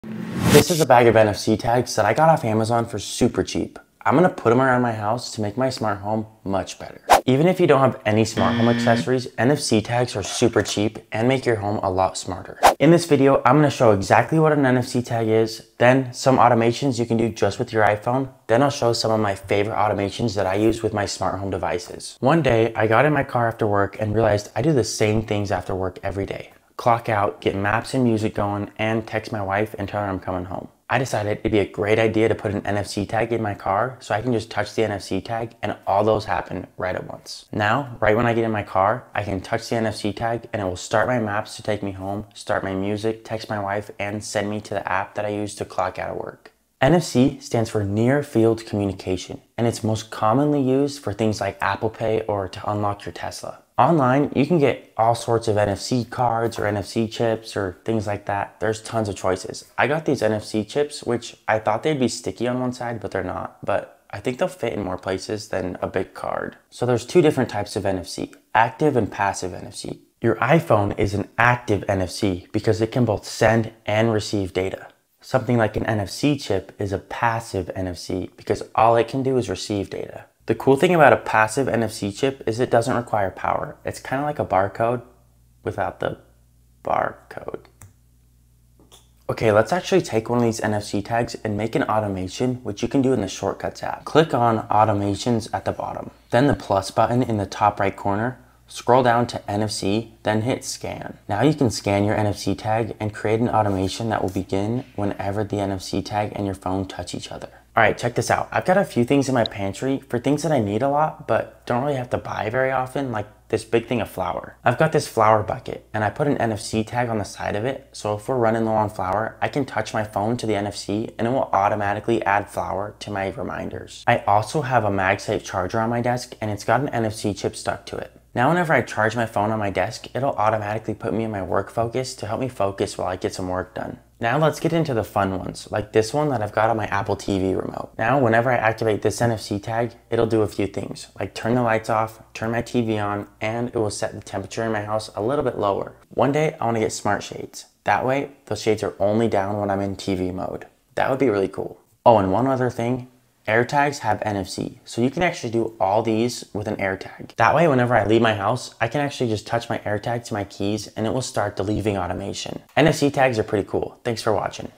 NFC Is The Most UnderRated piece of Technology
I am just doing these for fun but, let’s learn about HomeKit! 🍎

50 pack nfc tags: